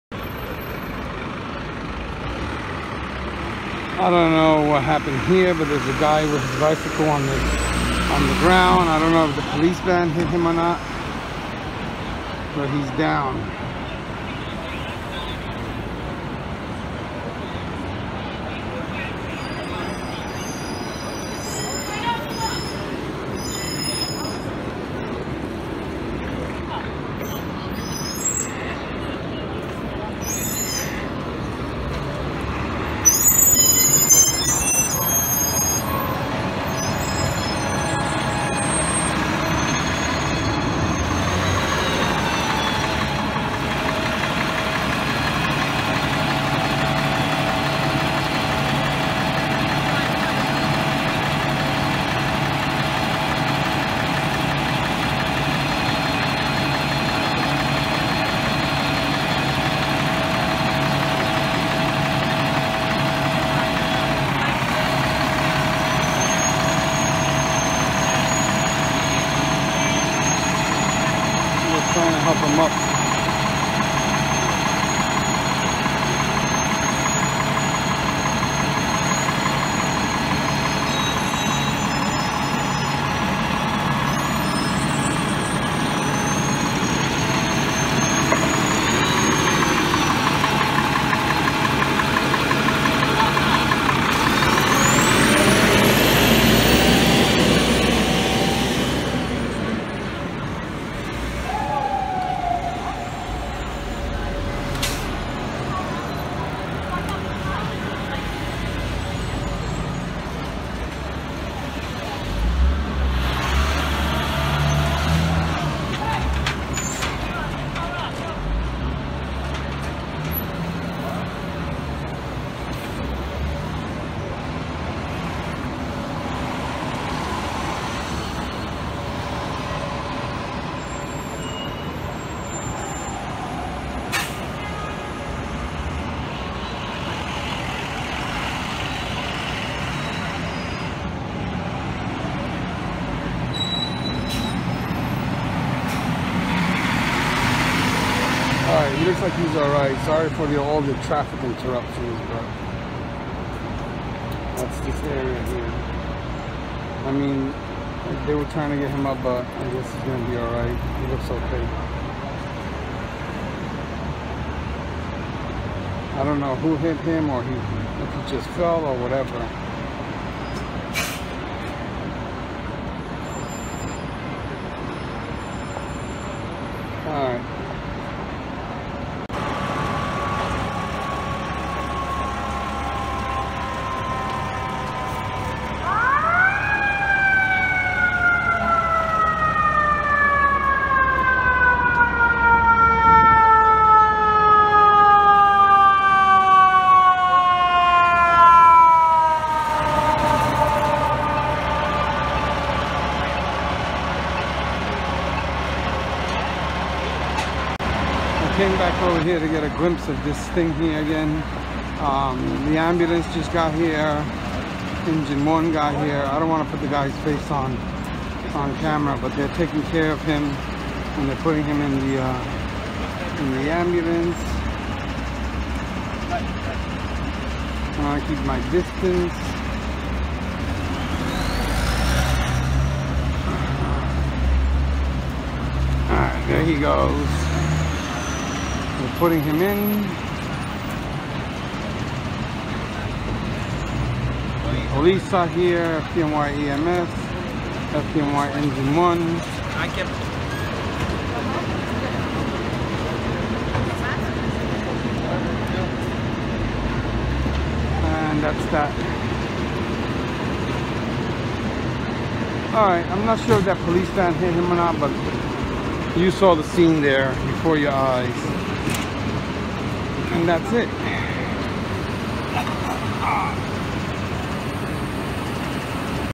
FDNY RESPONDING & ON SCENE OF PEDESTRIAN STUCK BY CAR ON W. 34TH ST. IN MANHATTAN, NYC.
HERE YOU WILL SEE THE FDNY RESPONDING AND ON SCENE OF A PEDESTRIAN STUCK BY A CAR ON WEST 34TH STREET IN THE MIDTOWN AREA OF MANHATTAN IN NEW YORK CITY.